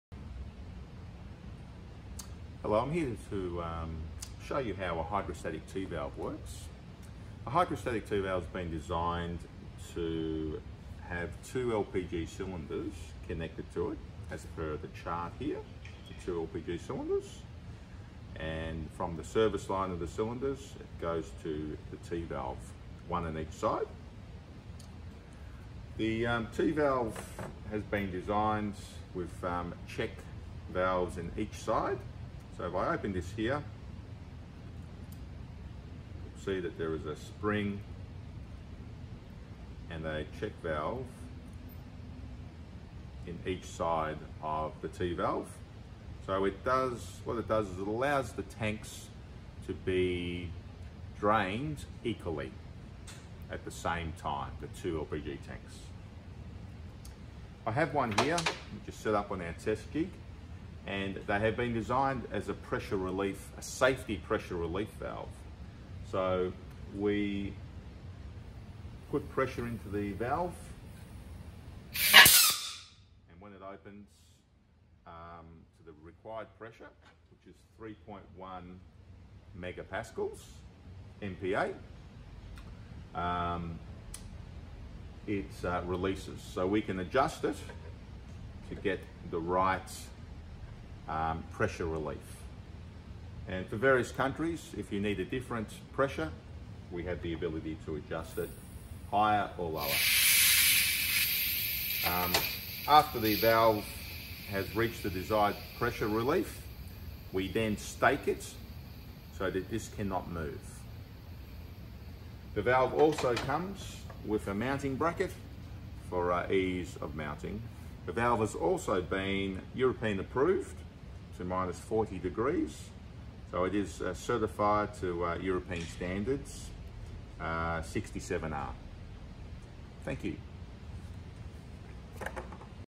Twin Dual LPG Tank Tee Valve
The Hydrostatic Tee Valve has been designed to have the service line of both LPG Tanks connect to it and then have one outlet its function is to open or release pressure if and when the combined pressure of both LPG Tanks exceeds 3.1Mpa according to the Countries relevant standards.